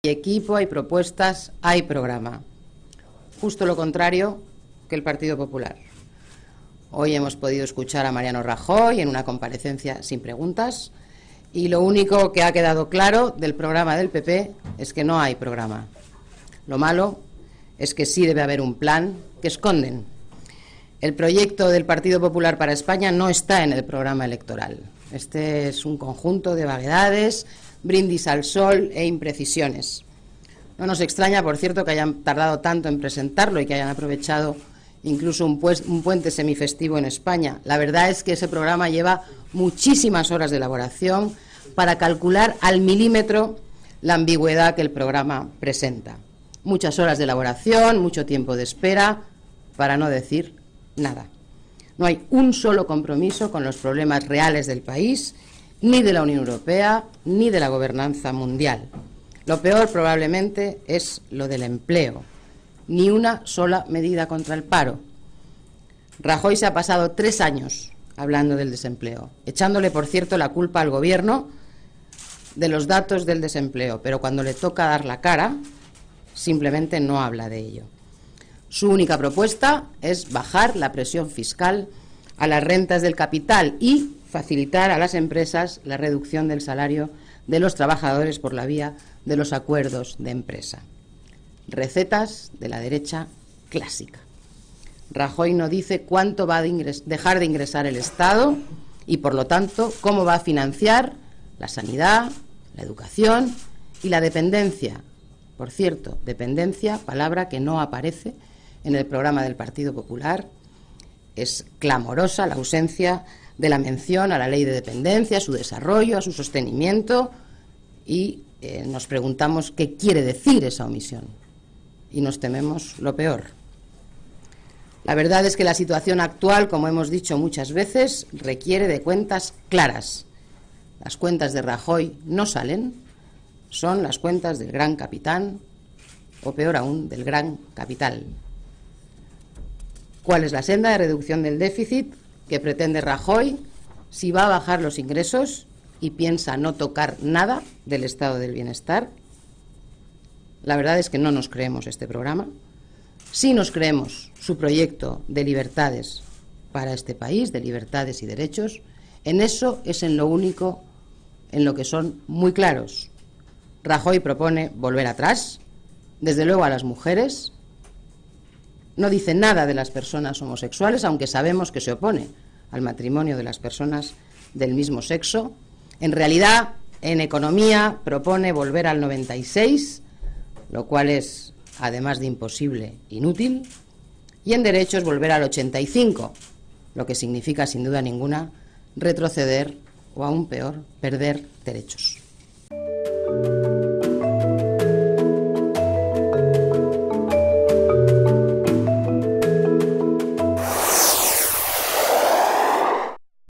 Elena Valenciano valora el programa electoral del PP
Redacción, 31 de octubre (CERESTV.info)

Valenciano denuncia que el PP quiere "en economía volver al 96, lo que además de imposible es inútil, y en derechos, volver al 85, lo que significa retroceder y perder derechos" .